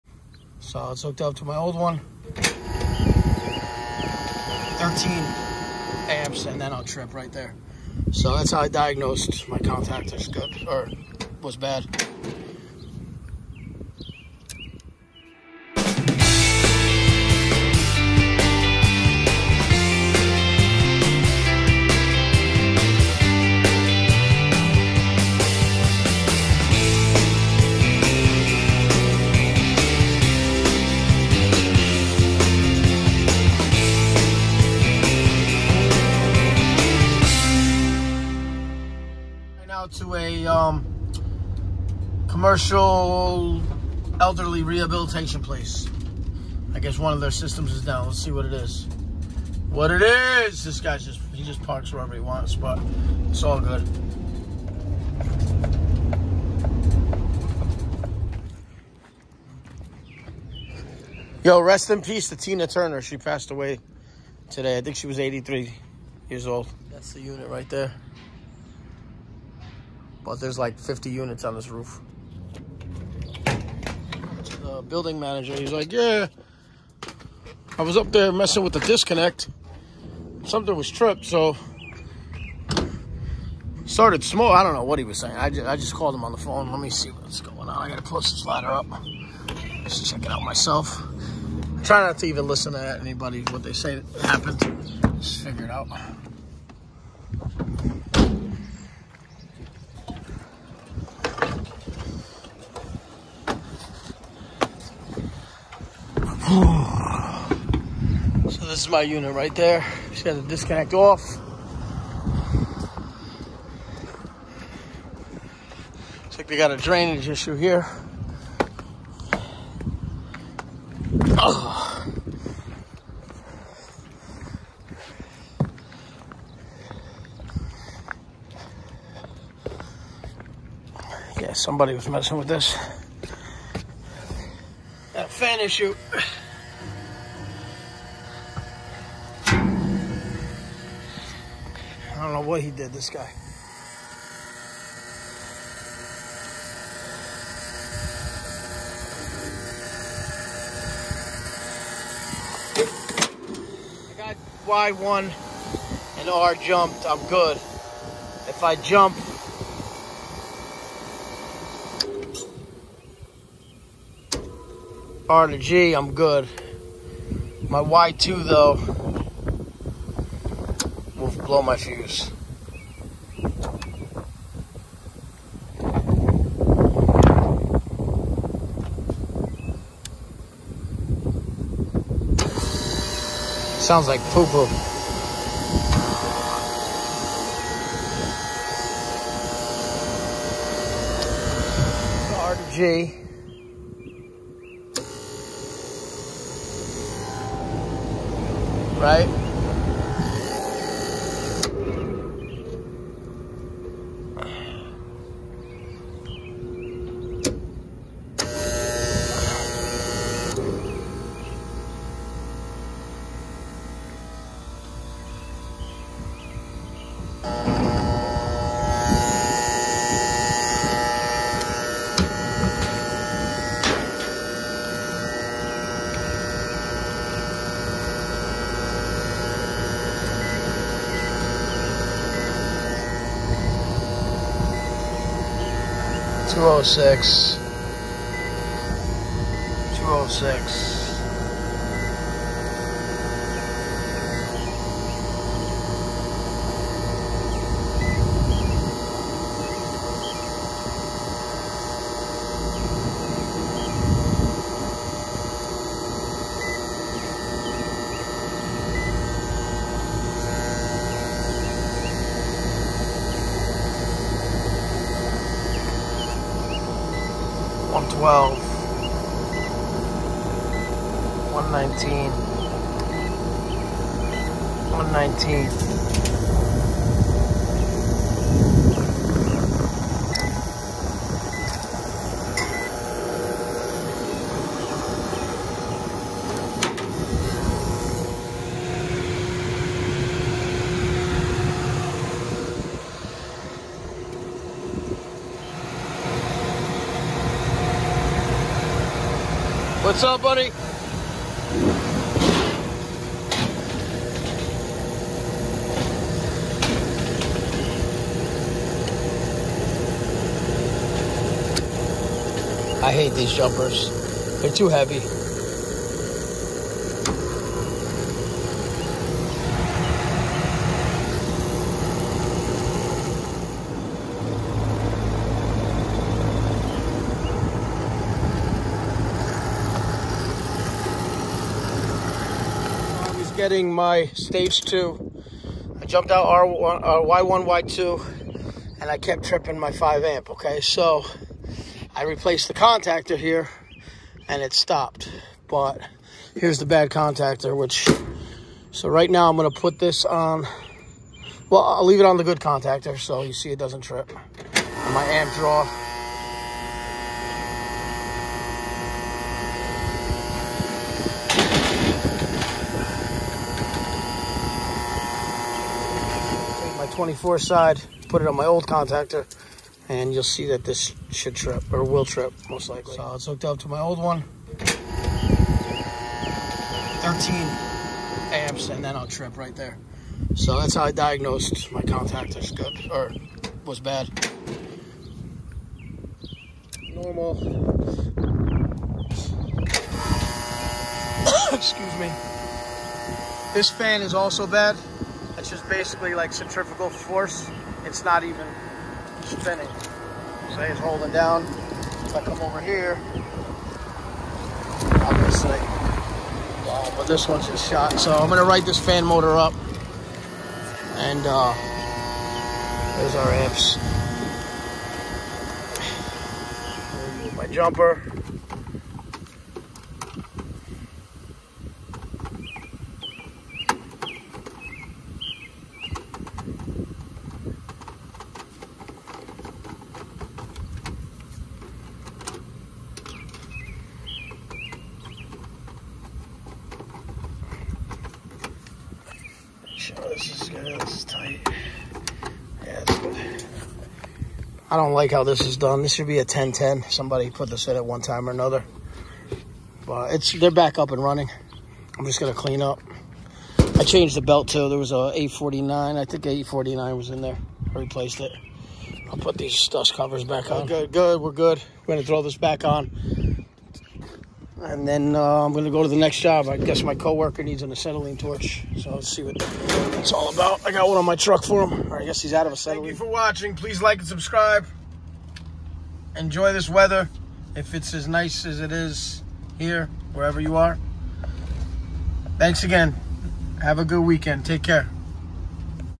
CARRIER NO AC. FUSE TRIPPING.
5 amp fuse on Carrier RTU tripping causing system to shut down.